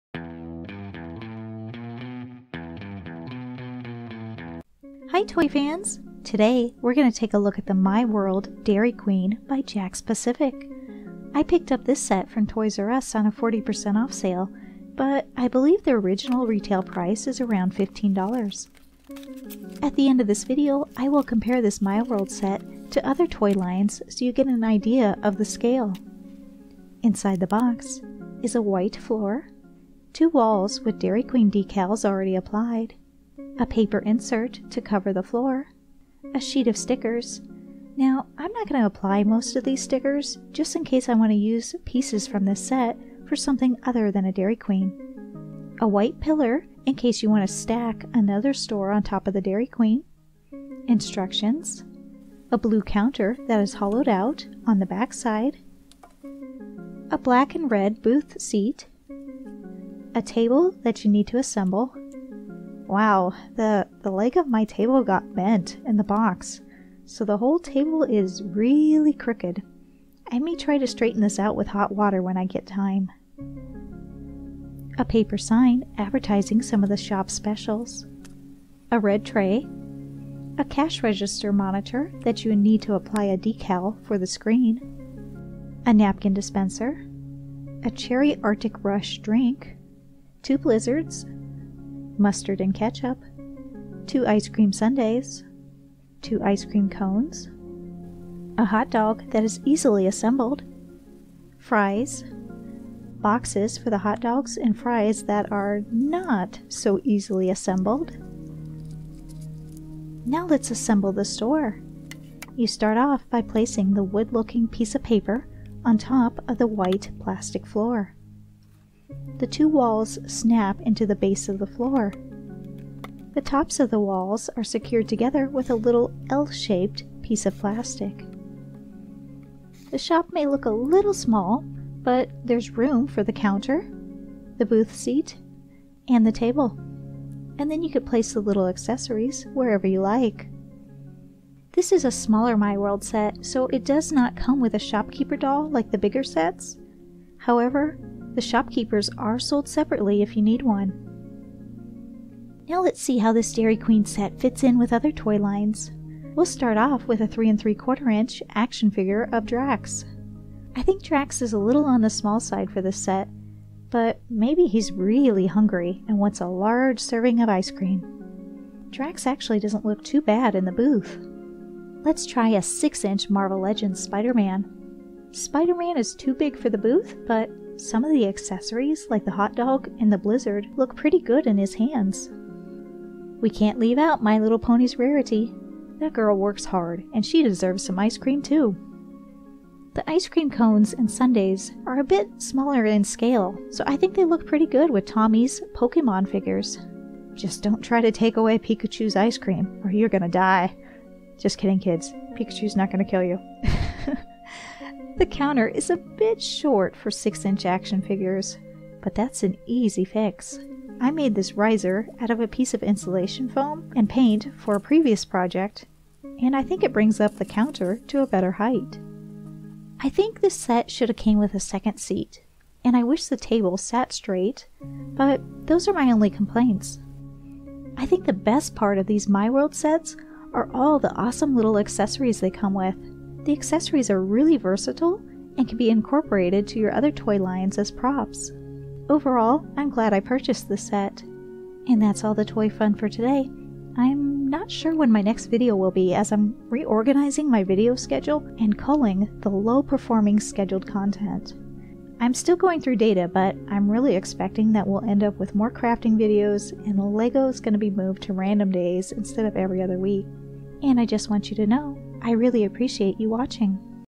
MiWorld Dairy Queen Store Review
Today we’re going to take a look at the MiWorld Dairy Queen by Jakks Pacific.  The original retail price for this MiWorld Store is around $15.

At the end of this video I will compare this Mi World set to other toy lines so you get an idea of the scale.

Inside the box is:
A white floor,
2 walls with Dairy Queen decals already applied,
A paper insert to cover the floor
A sheet of stickers
A white pillar in case you want to stack another store on top of the Dairy Queen
Instructions
A blue counter
A black and red booth seat
A table that you need to assemble
A paper sign advertising some of the shop’s specials
A red tray
A cash register monitor that you need to apply a decal for the screen
A napkin dispenser
A Cherry Arctic Rush drink
2 Blizzards
Mustard and ketchup
2 ice cream sundaes
2 ice cream cones
A hot dog that is easily assembled
Fries
Boxes for the hot dog and fries

To assemble the store, you start off by placing the wood looking paper piece on top of the white plastic floor.  The two walls snap into the base of the floor.  The tops of the walls are secured together with a little L shaped piece of plastic.

The shop may look a little small, but there is room for the counter, the booth seat, and the table.  Then you can place the little accessories wherever you like.

This is a smaller Mi World set so it does not come with a shopkeeper doll like the bigger sets.  However, the shopkeepers are sold separately if you need one.

In the video I compare this MiWorld Dairy Queen set to other toy lines such as 3.75 inch Marvel action figures, 6 inch Marvel Legends, My Little Pony (regular size brushable), Pokemon Ash, Tyrunt, and Pikachu.

I think this set should have came with a second seat, and I wish the table sat straight but those are my only complaints.  I think the best part of these Mi World sets are all the awesome little accessories they come with.  The accessories are very versatile and can be incorporated into your other toy lines as props.  Overall, I’m glad I purchased this set.

If you enjoyed this review, you should check out the super cute MiWorld Pet Shop